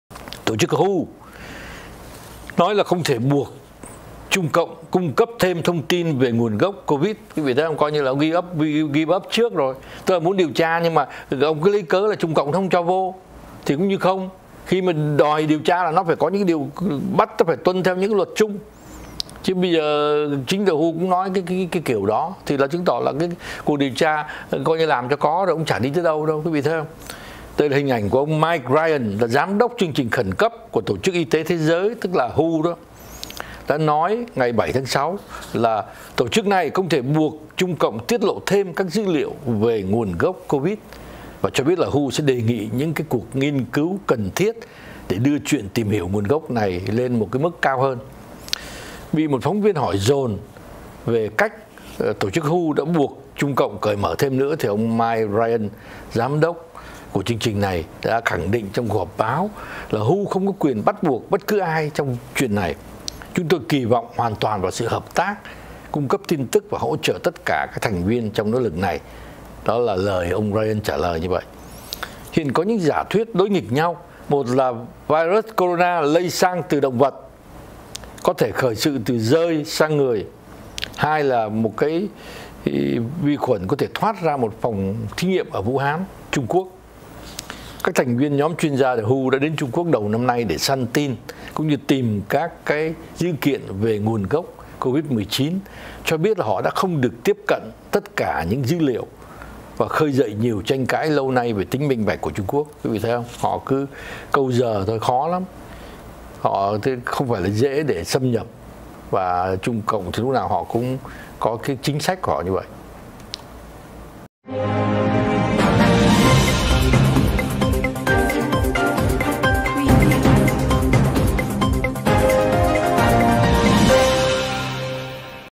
WHO không thể buộc Trung Quốc cung cấp thêm thông tin về nguồn gốc Covid-19
Ông Mike Ryan, giám đốc chương trình khẩn cấp của WHO cho biết tổ chức này không thể buộc Trung Quốc tiết lộ thêm dữ liệu về nguồn gốc Covid-19 và cho biết WHO sẽ đề nghị những cuộc nghiên cứu cần thiết để đưa chuyện tìm hiểu về nguồn gốc của đại dịch lên "mức kế tiếp".
Trước đó, các thành viên trong nhóm chuyên gia của WHO đã đến Trung Quốc để tìm nguồn gốc về Covid-19. Tuy nhiên họ cho biết không được tiếp cận tất cả dữ liệu. Đây cũng là điều gây ra nhiều tranh cãi về tính minh bạch của nước này.